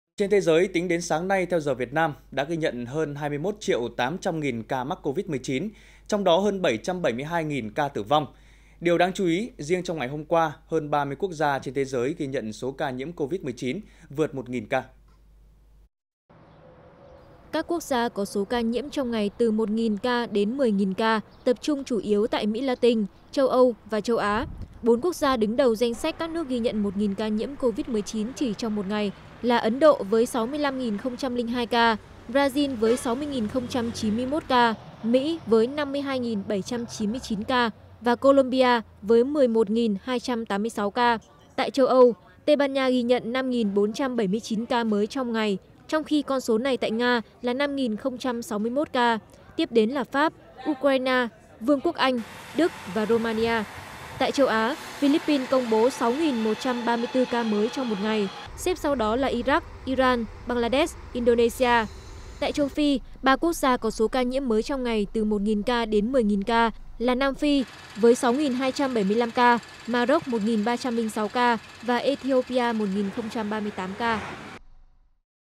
Hơn 30 quốc gia có số ca nhiễm Covid-19 trong ngày vượt 1.000 ca
Hơn 30 quốc gia có số ca nhiễm Covid-19 trong ngày vượt 1.000 ca | Truyền Hình Nhân Dân
1. Dự Báo Thời Tiết Hôm Nay